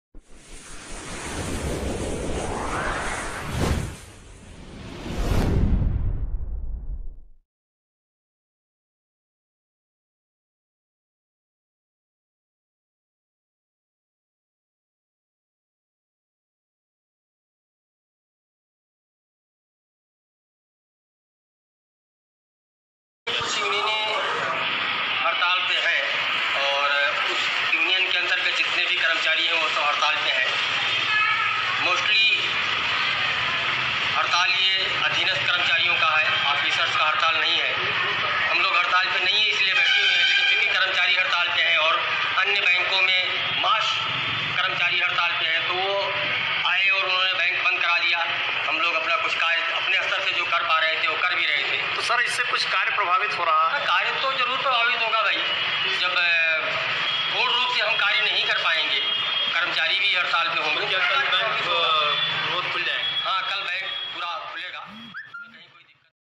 गोण्डा जनपद में बैंक के अधीनस्थ कर्मचारियों की हड़ताल के कारण लेनदेन हुआ प्रभावित।
डॉ0कल्पराम त्रिपाठी
ब्यूरोचीफ गोण्डा

गोण्डा जनपद में दीपावली का पर्व नजदीक होने के कारण इन दिनों लगभग बैंकों पर उपभोक्ताओं की भारी भीड़ जुट रही है l आज कुछ बैंकों के हड़ताल होने के कारण बैंक से जमा निकासी करने आए ग्राहकों को काफी कठिनाइयों का सामना करना पड़ा।जिससे वह मायूस होकर घर लौट गए। प्रमुख बैंकों में पंजाब नेशनल बैंक, सेंट्रल बैंक, इलाहाबाद बैंक, के अधीनस्थ कर्मचारी बैंक का एंप्लाइज यूनियन के आह्वान पर हड़ताल पर रहे lहालांकि बैंक के अधिकारियों का कहना है अधिकारी संवर्ग के लोग हड़ताल पर नहीं हैं l केवल अधीनस्थ बैंक कर्मचारी हड़ताल पर हैं ।फैसले में आज अधीनस्थ कर्मचारियों ने बैंक पर तालाबंदी बंदी कर दी l जिससे कामकाज काफी प्रभावित हुआ है । अधिकांश उपभोक्ता बैंक के दरवाजे से मायूस होकर लौट गए
बैंक के अधिकारी भी इस बात की पुष्टि करते हैं , की अधीनस्थ कर्मचारियों की हड़ताल की वजह से लेन देन काफी हद तक प्रभावित हुआ है l
फिर भी हम लोगों द्वारा जो कुछ किया जा सका वह किया जा रहा है l